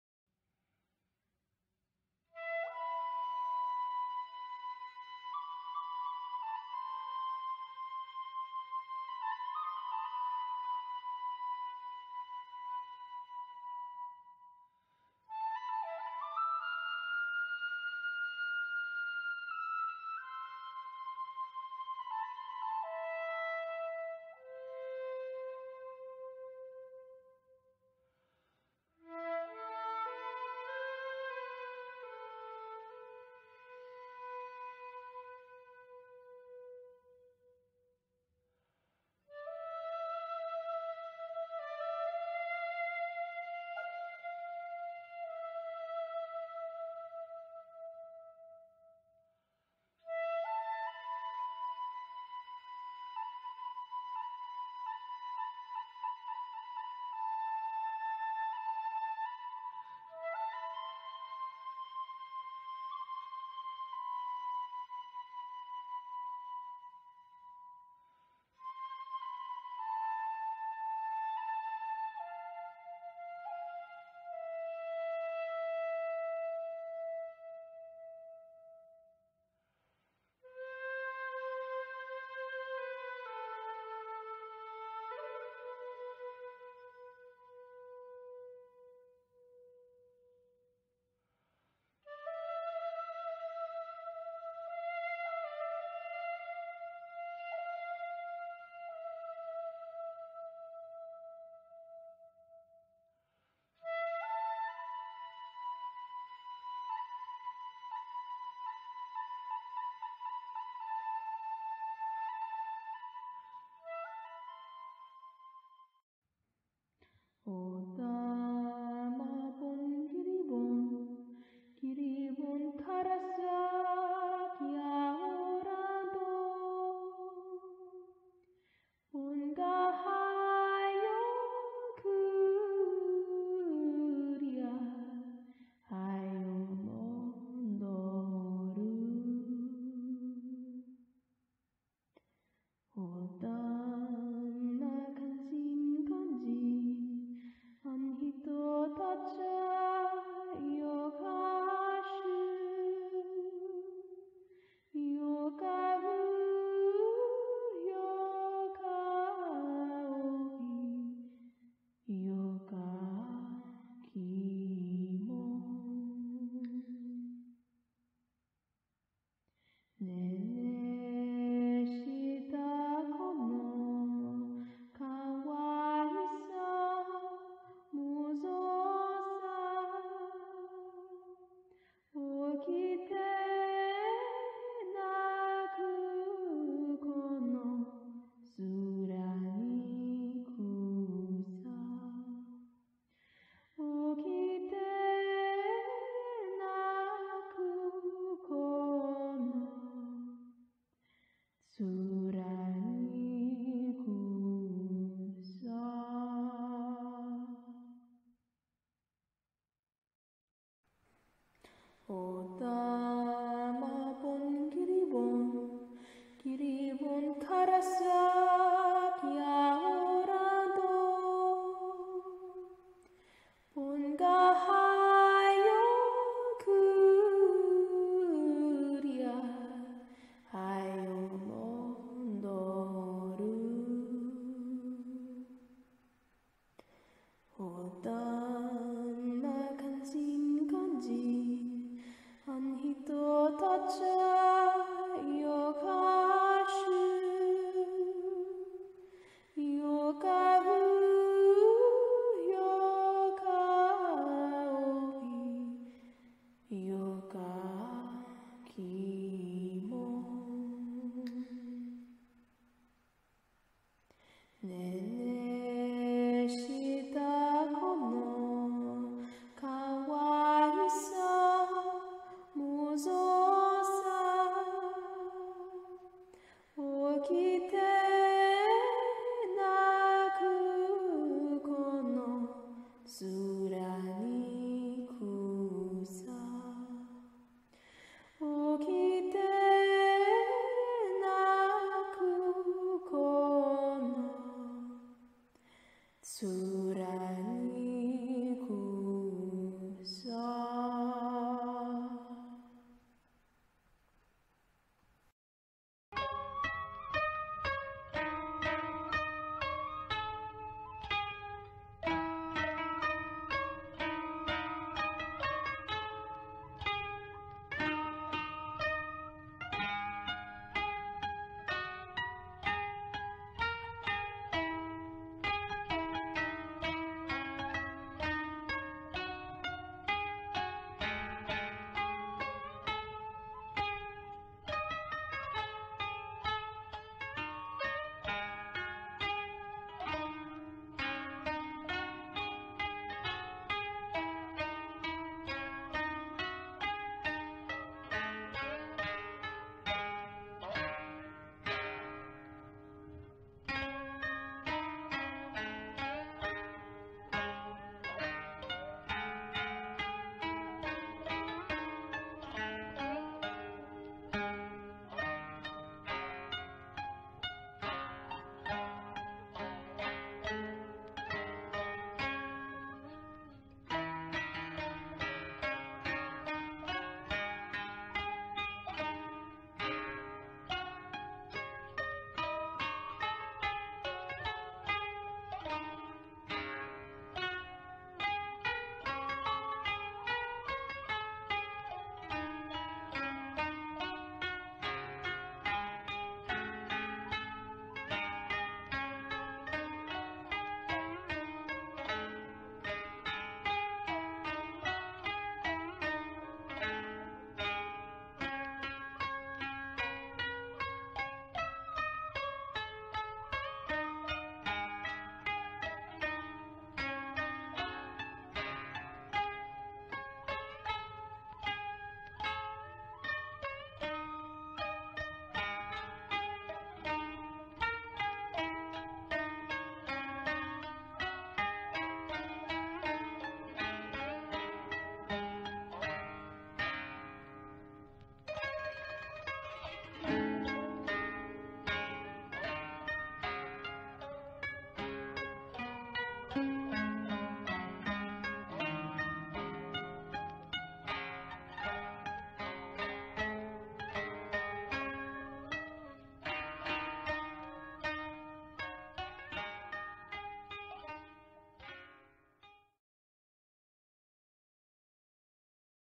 Slide Show of Photos with Mitsu Suzuki
Mitsu Suzuki is the widow of Shunryu Suzuki known as Suzuki Roshi, founder of the San Francisco Zen Center. or Shunryu Suzuki dot com.